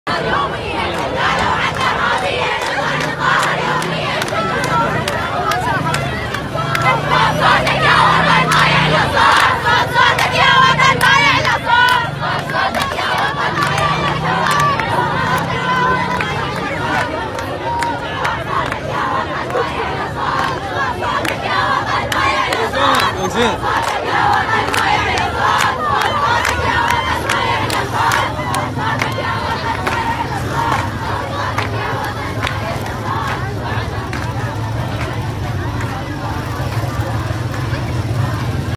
طلبة قضاء الشطرة بمحافظة ذي قار يؤكدون استمرار الإضراب العام دعمًا لـ
ــــــــــــــــــــــــــــ